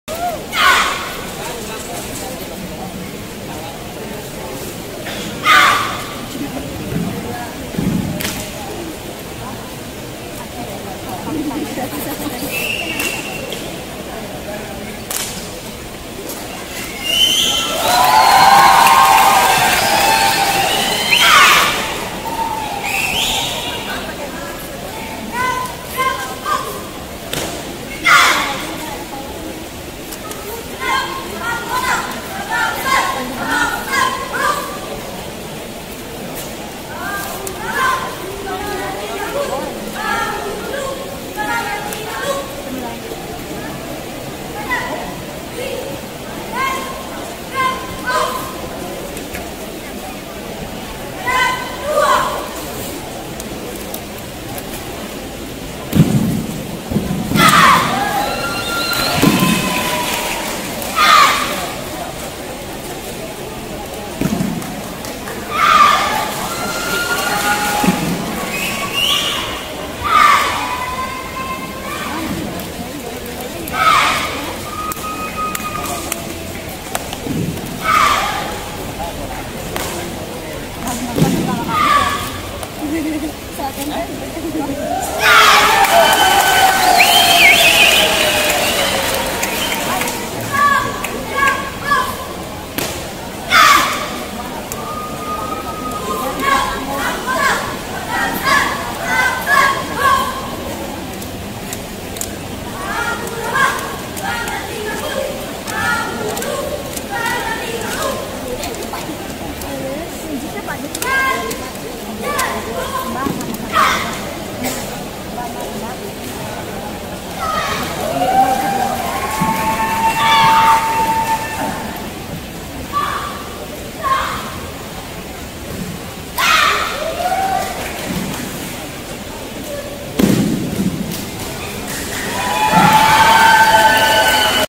Seni Gerak (RANGER PUTRI) ACEH PRA-PON 2019
Seni Gerak (RANGER PUTRI) ACEH pada Kejuaraan Pra-Kualifikasi PON di Jawa Barat, 2019